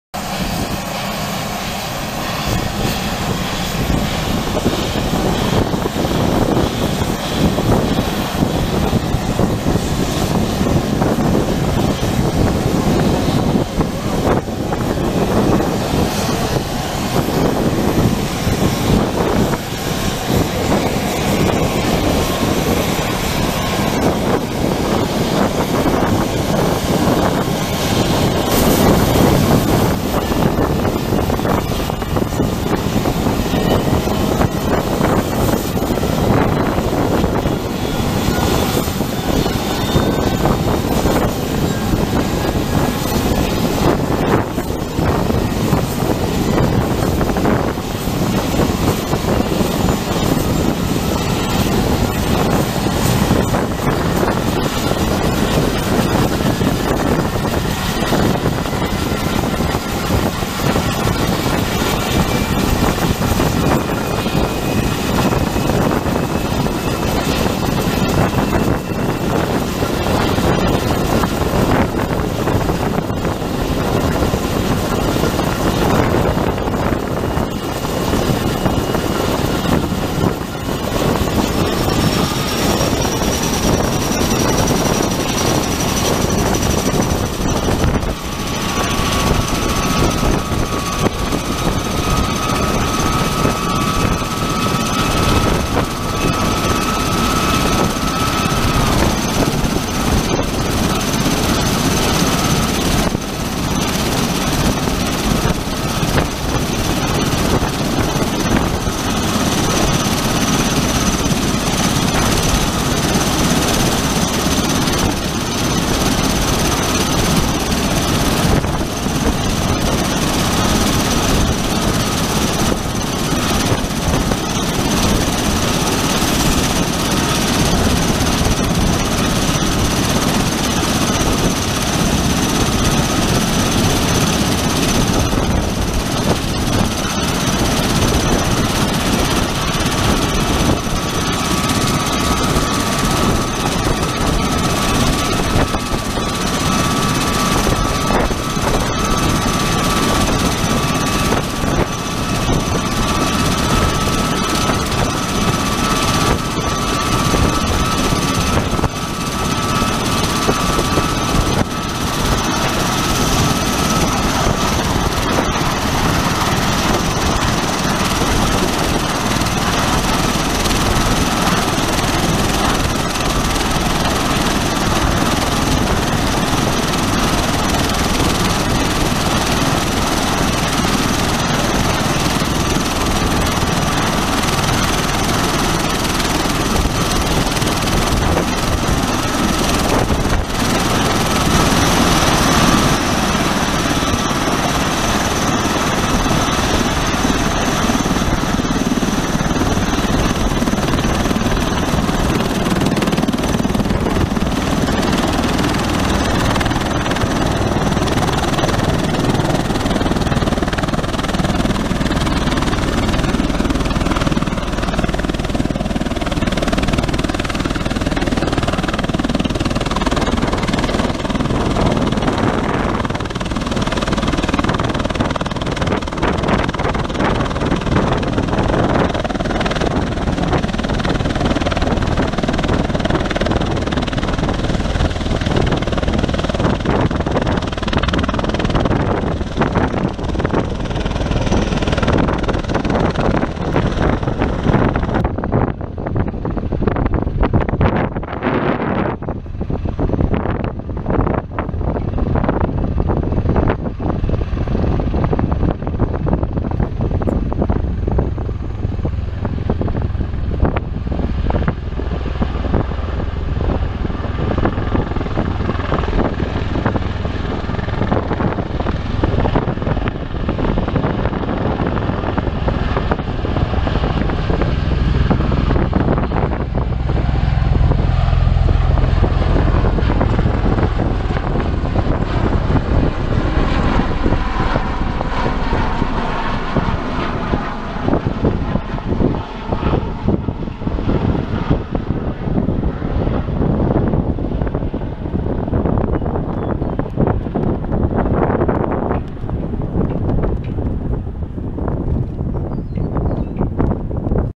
MV-22B Osprey takes off from Owatonna Degner Regional Airport
An MV-22B Osprey lifts off from Owatonna Degner Regional Airport shortly before 9 a.m. Wednesday, June 3, after an overnight stop to refuel and for the crew to perform some recruitment activities.